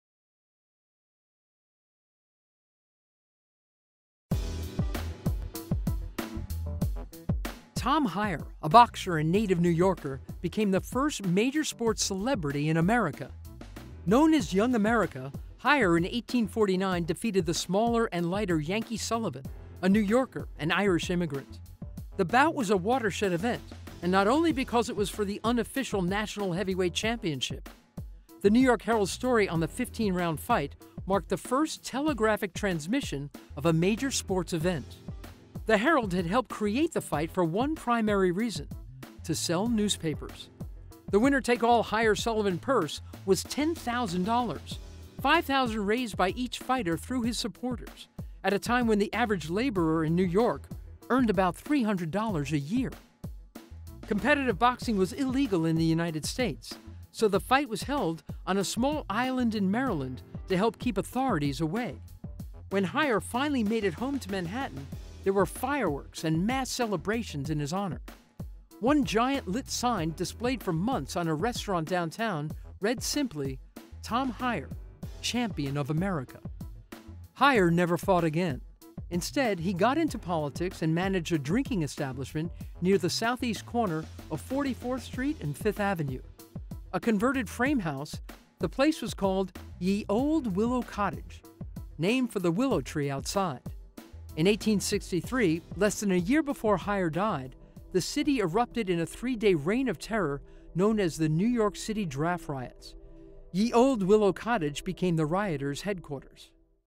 Tom Hyer: A 'New York Game Changers' Story
This story about boxer Tom Hyer is from the original series 'New York Game Changers' by New York Sports Experience.

Each 'New York Game Changers' story is tied to a Manhattan address. The location of the Hyer story is 527 Fifth Avenue, where Ye Ole Willow Cottage (mentioned in the story) stood.

One block to the west of the address is the location for the 'New York Game Changers' story 'The Hippodrome.' Two blocks northeast of 'Tom Hyer' is the location for the stories 'Civil Rights' and 'Roger Donoghue.'